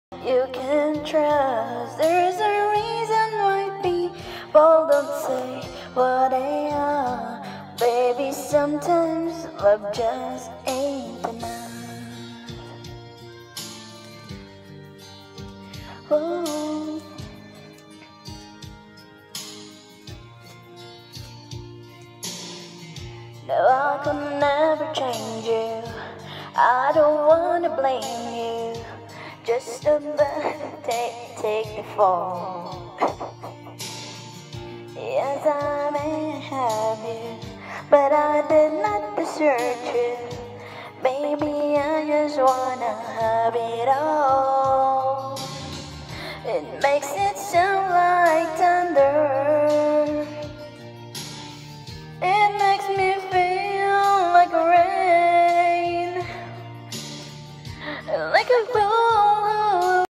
6 months old baby boy holds the mic for his momma_ supportive son
He actually held the mic for me while I'm singing, he looks so Inlove to momma although my voice is not good lol